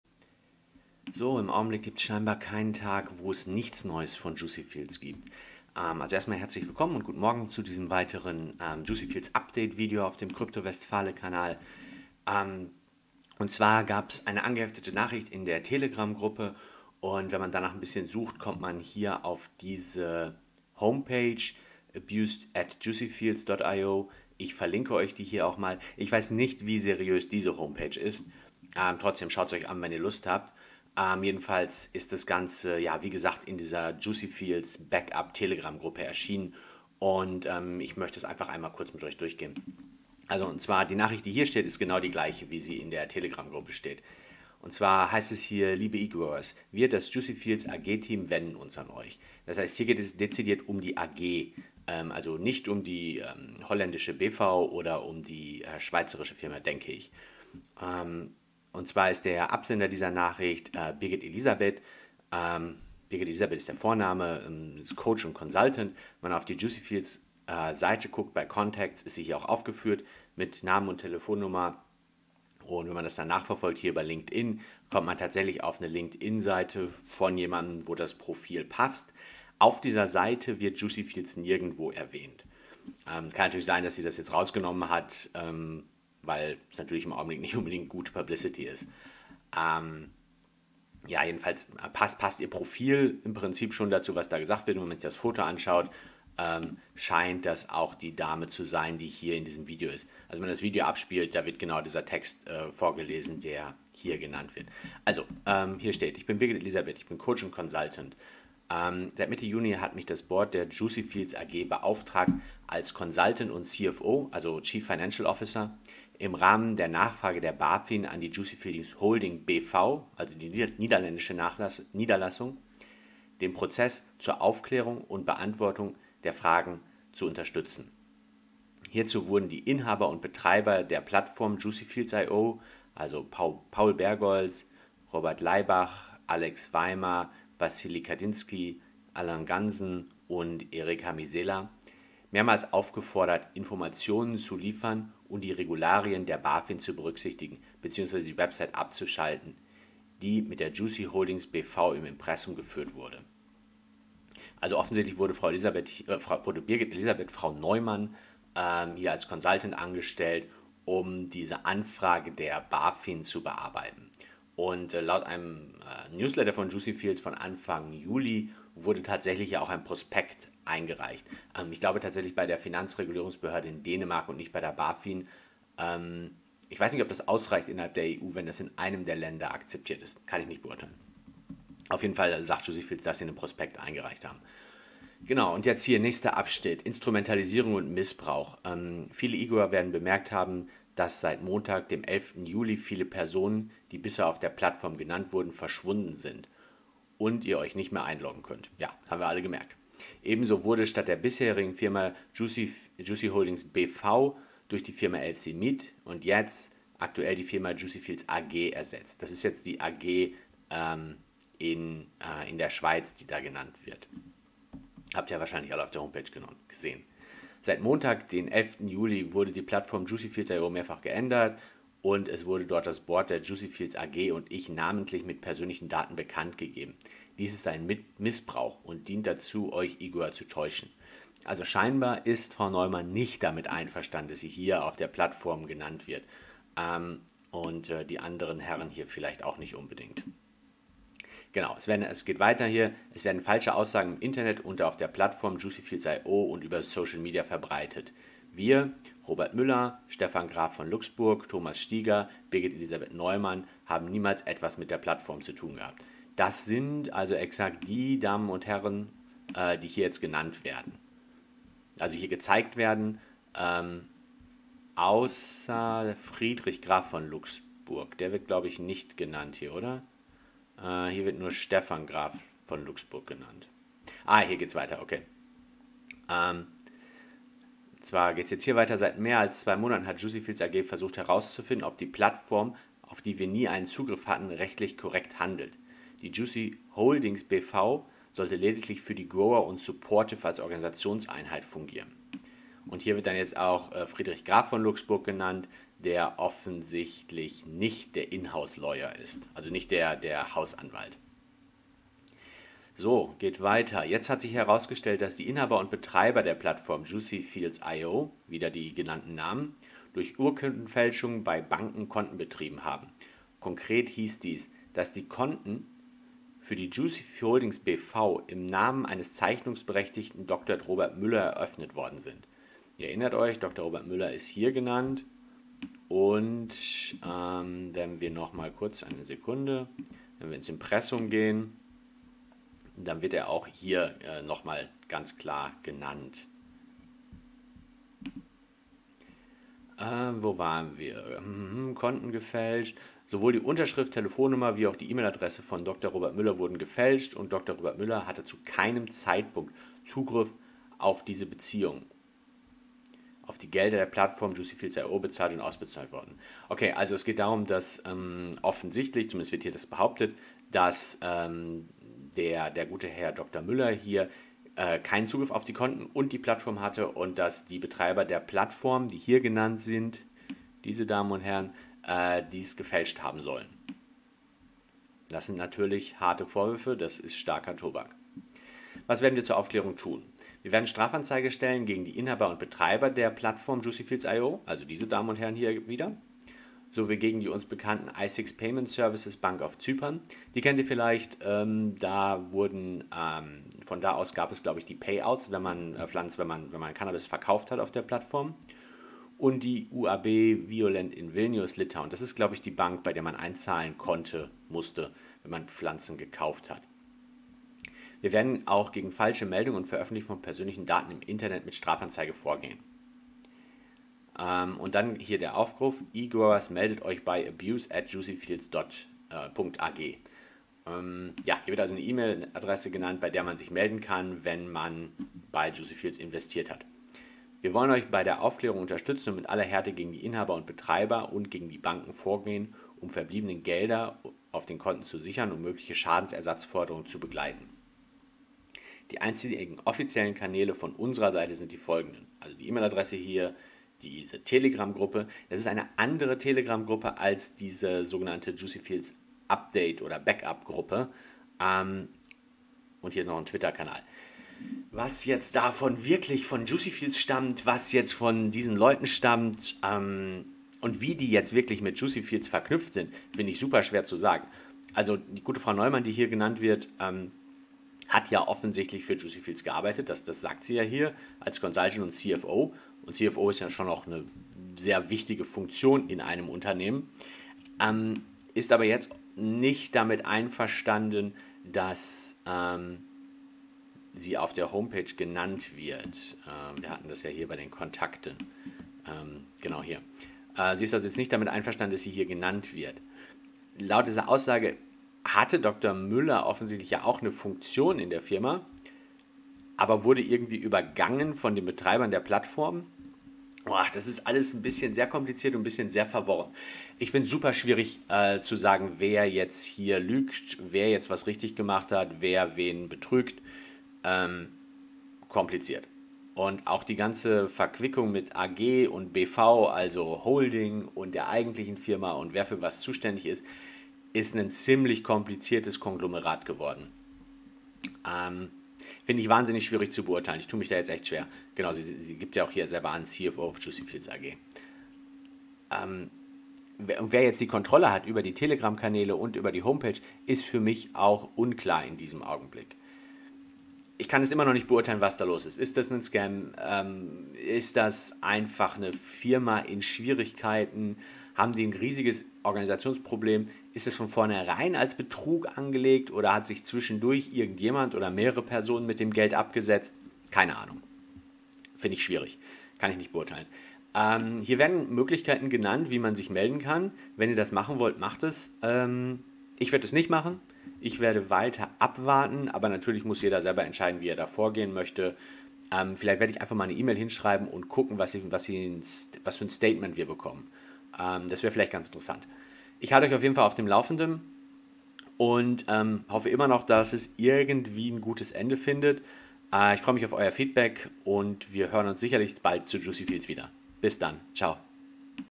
Juicy Fields – Nachricht vom CFO. Krasse Anschuldigungen. Wer lügt hier?
Upate zu Juicy Fields mit einem Statement der CFO Birgit-Elisabeth Neumann.

👉 Hier auch noch meine Hörbuch-Empfehlung des Monats (Kostenlos bei Audibel): Kryptowährungen von Dr. Julia Hosp